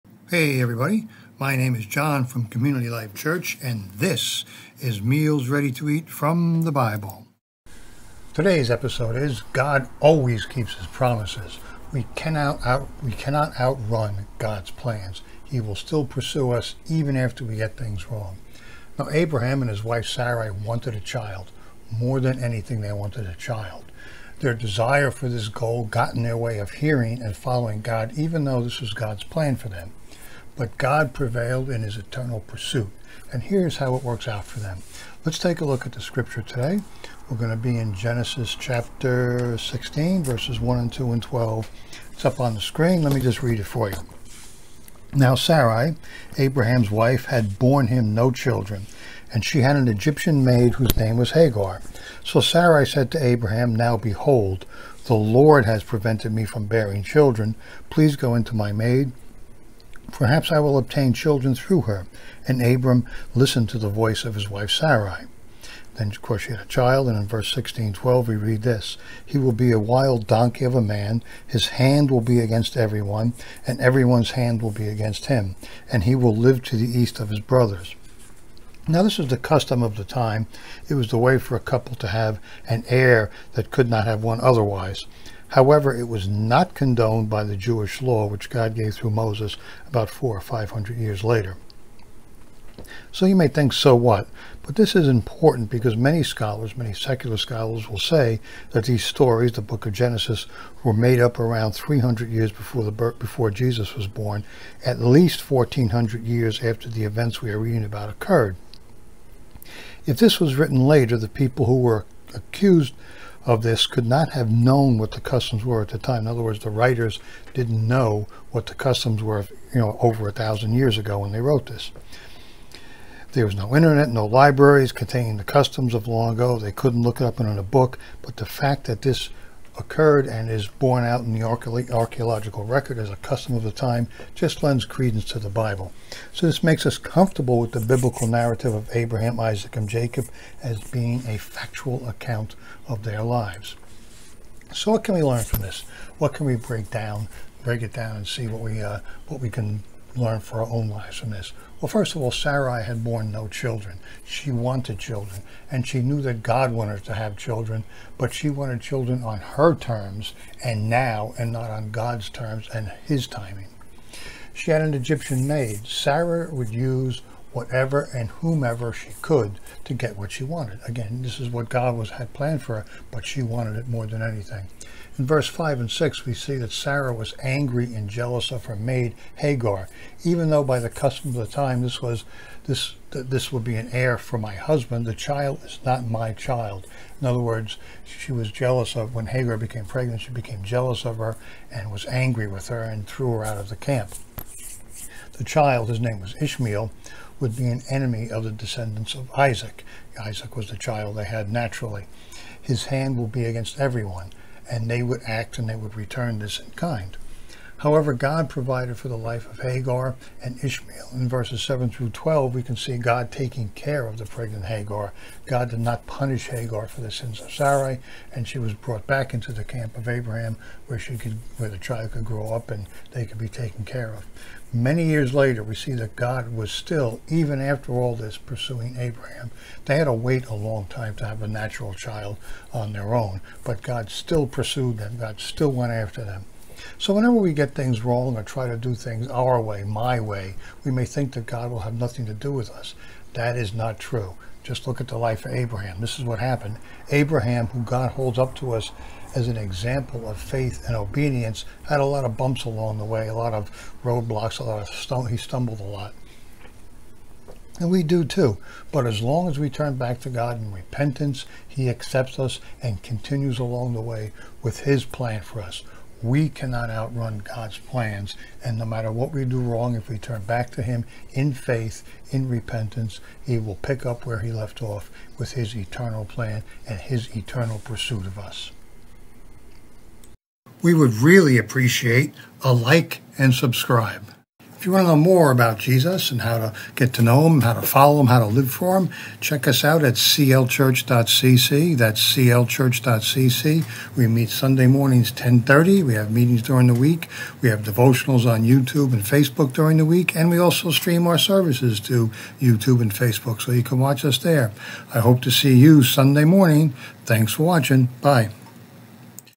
God ALWAYS keeps His promises.
We cannot outrun God’s plans!  He will still pursue us even after we get things wrong.